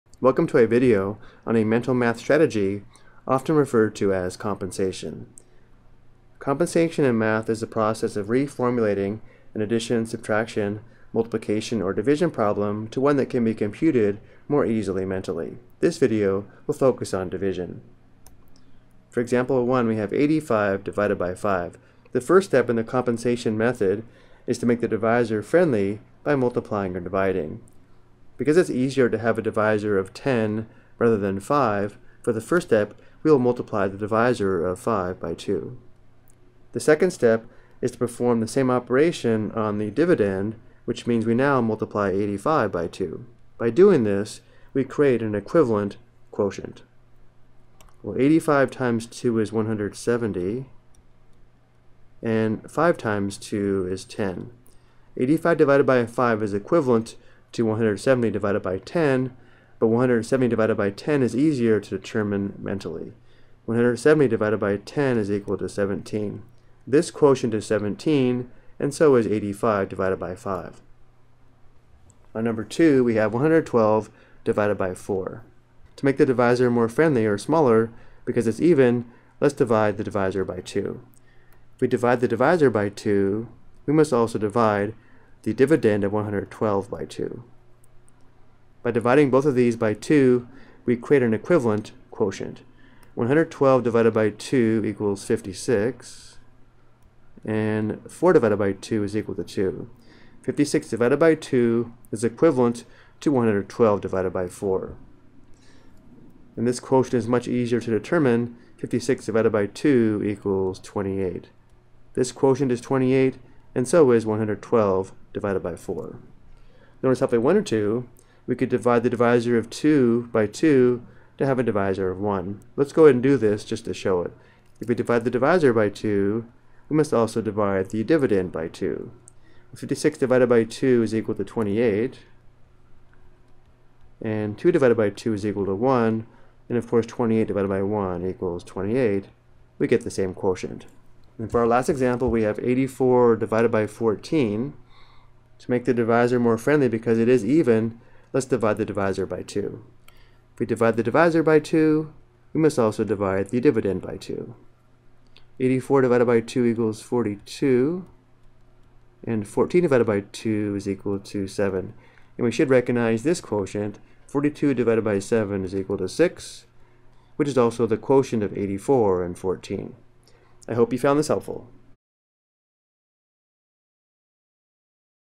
Mental Math: Division Using Compensation
This video explains how to use the method of compensation to help perform division mentally.